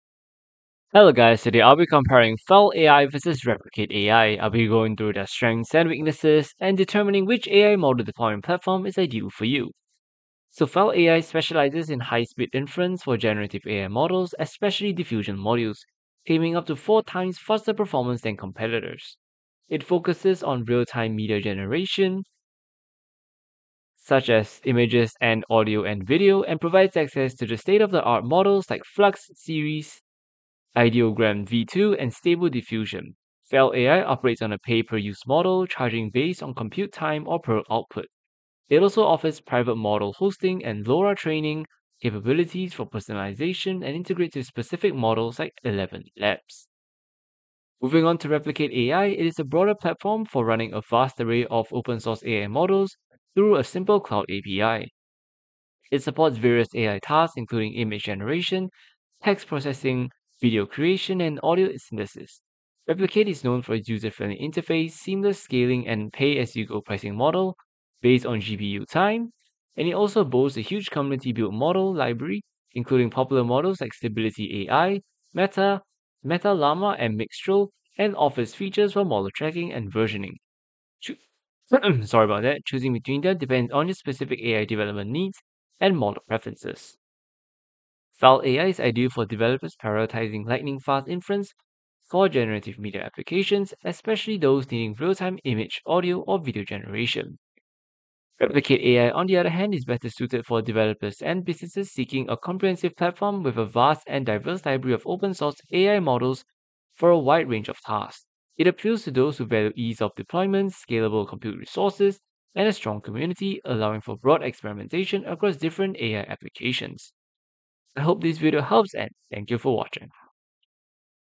Fal.AI vs. Replicate AI Review 2025 | Which AI Model Deployment Platform is more Worth It?
Fal.AI vs. Replicate AI: Which AI model deployment platform is best in 2025? We compare lightning-fast generative media with a vast open-source model library!